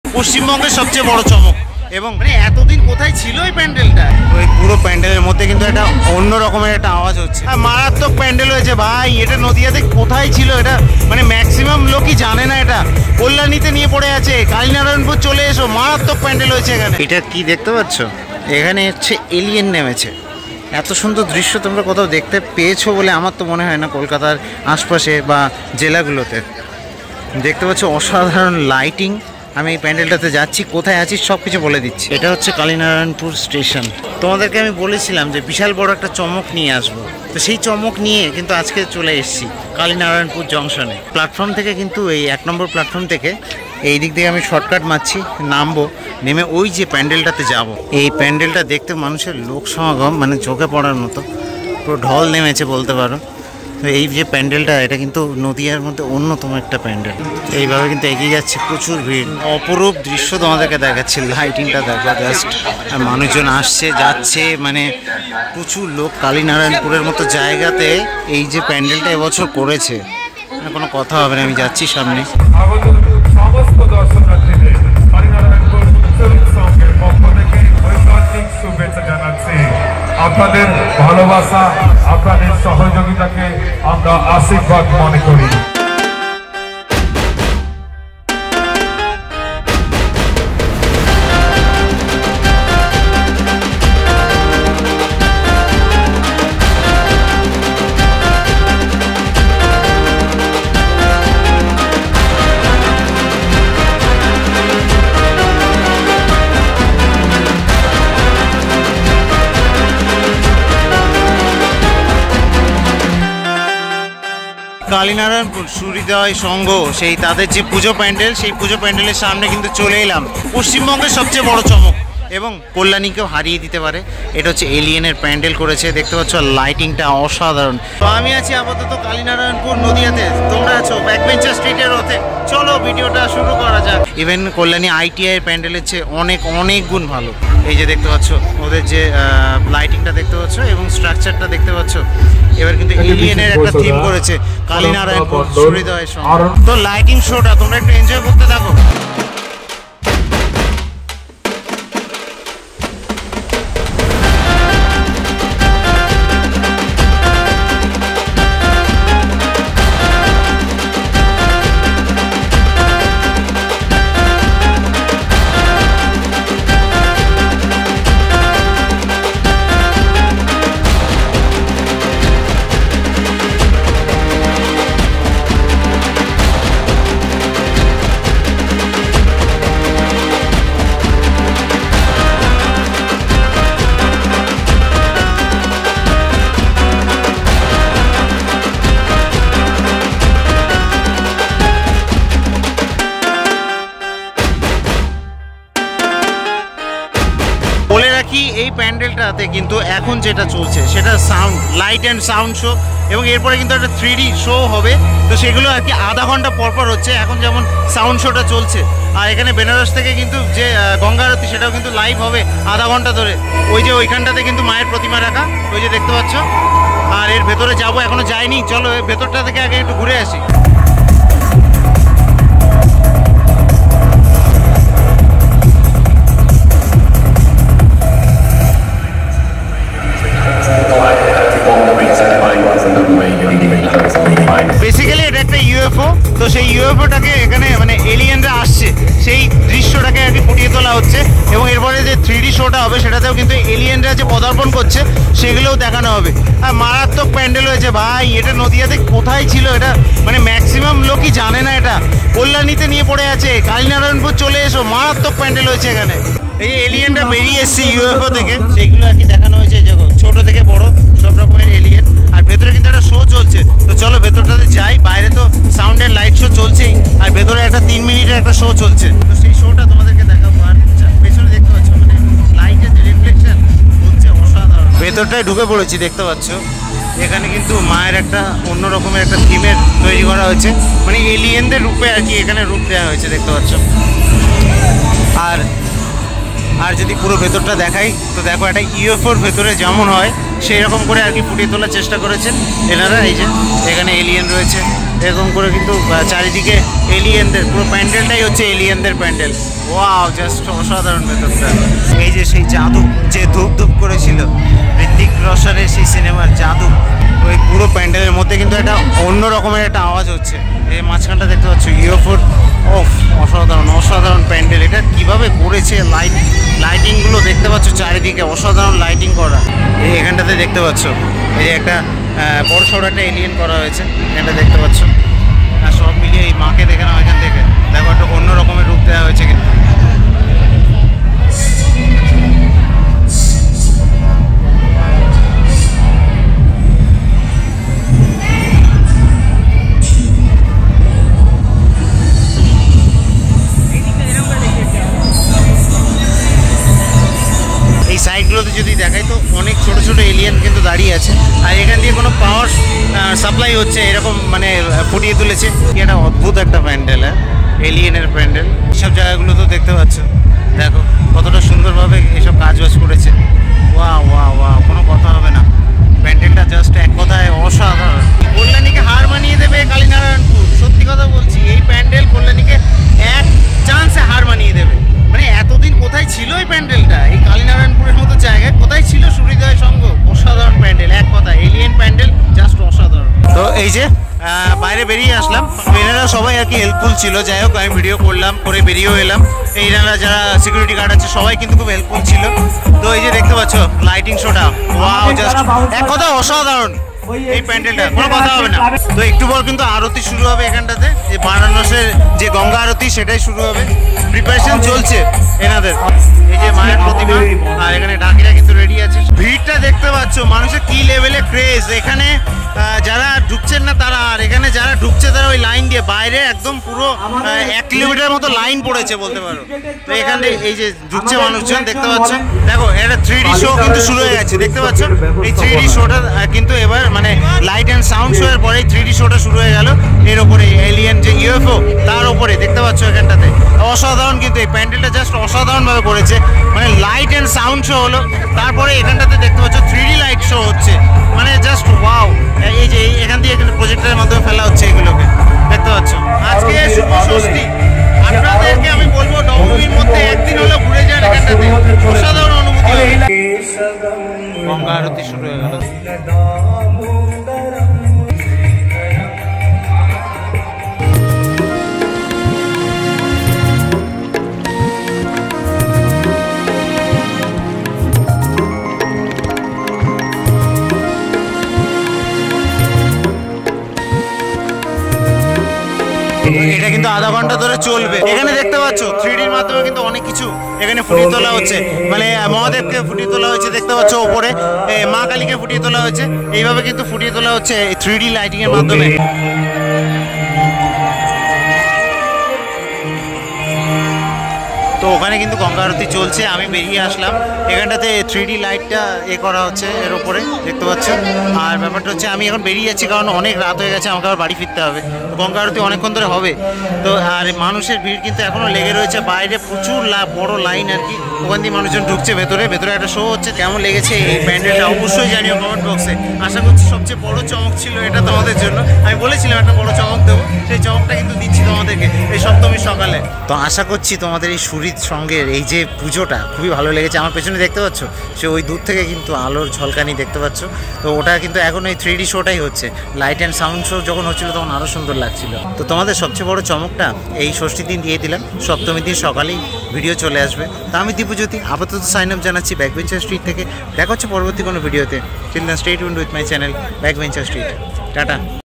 দূর্গাপূজোর সবচেয়ে বড়ো চমক🔥🔥এলিয়েন প্যান্ডেল ঘুরলাম ll Kalinayrayanpur Durga Puja 2023
Welcome back to another video by Backbencher Street of Durga puja 2023. In this video I will show you the best durga puja pandal in west bengal in terms of lighting. This Kalinarayanpur durga puja 2023 pandal will give competition to the biggest kolkata durga puja 2023 pandal. Gemerally this kind of durga puja is not often seen in districts as it can consume more budget for the durga puja committe. In my openion Santosh mitra square of lebutala park from kolkata durga puja 2023 is the best lighting and theme pandal in Kolkata but after that this pandal will capture the tag of best durga puja pandal. This allien pandal is created in such a awesome way where they have given light and sound show outside for 5 minutes and inside there are also a 3 min show. Ganga arti is also performed here in kalinarayanpur suhrid sangha durga puja. Hopefully you will enjoy the allien pandal. I have visited many palces for durga puja in nadia such as badkulla durga puja 2023, taherpur durga puja 2023, ranaghat durga puja 2023, kalyani durga puja 2023 and now i gave come to Kalinarayanpur to witness Kalinarayanpur durga puja 2023. There is only one big pandal in this Kalinarayanpur junction station and this one is Kalinarayanpur Subrid sangha durga puja.